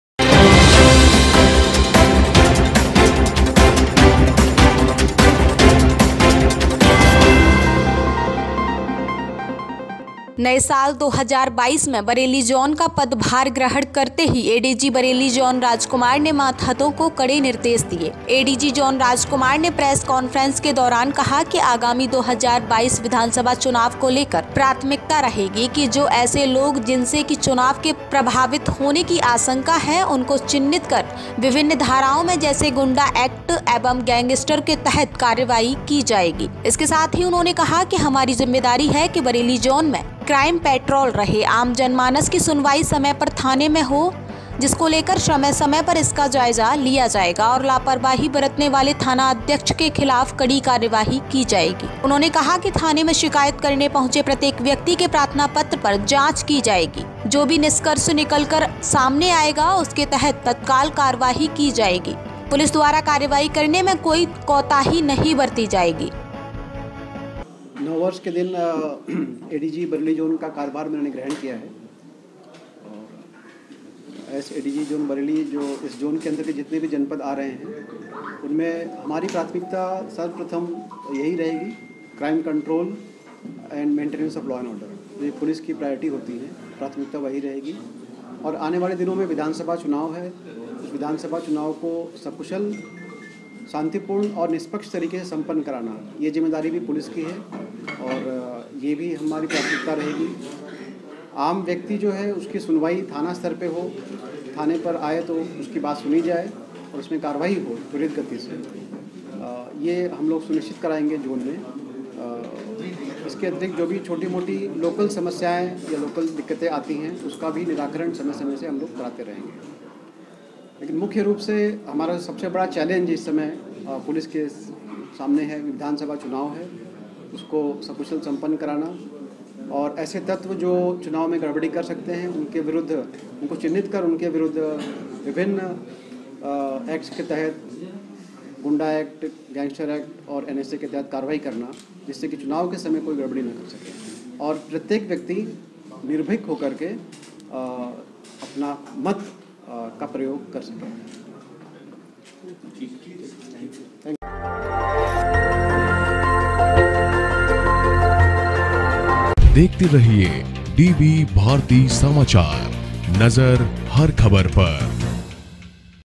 आगामी विधानसभा चुनाव 2022 को लेकर एडीजी जोन का फोकस/BAREILLY NEWS/04-01-22
आगामी विधानसभा चुनाव 2022 को लेकर एडीजी जोन का फोकस